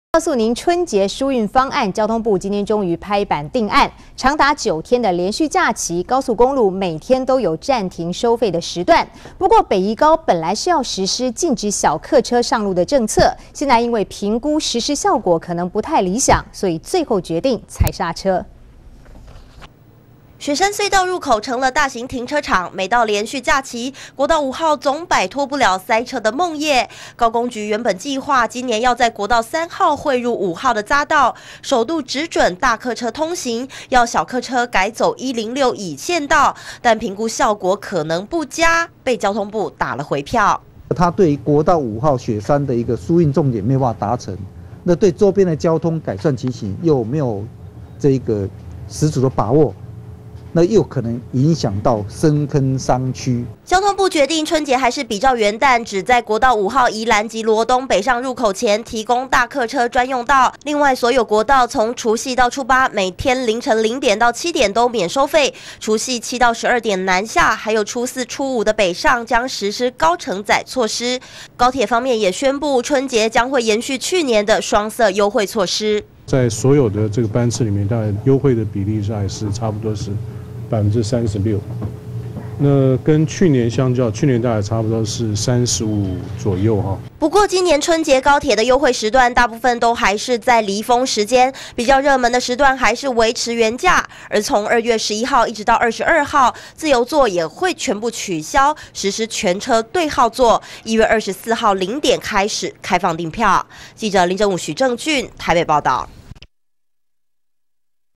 2010-01-21公視晚間新聞(春節疏運國道三號匯五號 小客車也可通行)
"春節疏運方案、交通部今天拍板定案。長達九天的連續假期、高速公路每天都有暫停收費時段!不過、北宜高本來要實施、禁止小客車上路的政策、現在因為評估實施效果可能不理想，最後決定踩煞車。
雪山隧道入口成了大型停車場，每到連續假期，國道五號總擺脫不了塞車的夢靨，高公局原本計畫，今年要在國道三號匯入五號的匝道，首度只准大客車通行，要小客車改走106乙縣道，但評估效果可能不佳，被交通部打了回票。
交通部決定春節還是比照元旦，只在國五宜蘭及羅東北上入口前，提供大客車專用道，另外所有國道從除夕到初八，每天凌晨零點到七點都免收費，除夕7到12點南下，及初四初五北上，將實施高乘載措施。高鐵部分，也宣布春節會延續去年的雙色優惠措施。
不過今年春節的優惠時段，大部分都還是離峰時間，比較熱門的時段還是維持原價，而從二月11號一直到22號，自由座也會全部取消，實施全車對號座，一月24號零點起開放訂票。
記者林珍汝許政俊台北報導。"